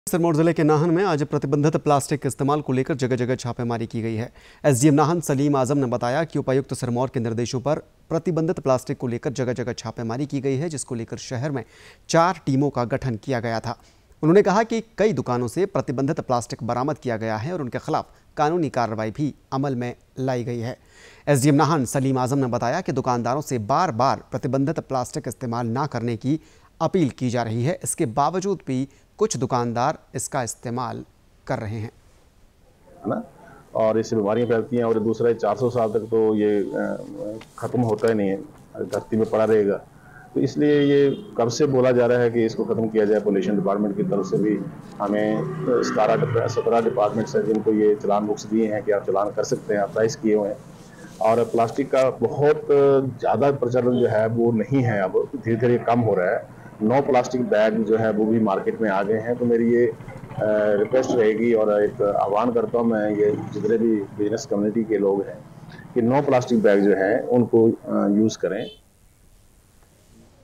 सिरमौर : प्रतिबंधित प्लास्टिक को लेकर नाहन में छापेमारी, दुकानों में पाया गया प्रतिबंधित प्लास्टिक।
सिरमौर : प्रतिबंधित प्लास्टिक को लेकर नाहन में छापेमारी, कई दुकानों में पाया गया प्रतिबंधित प्लास्टिक।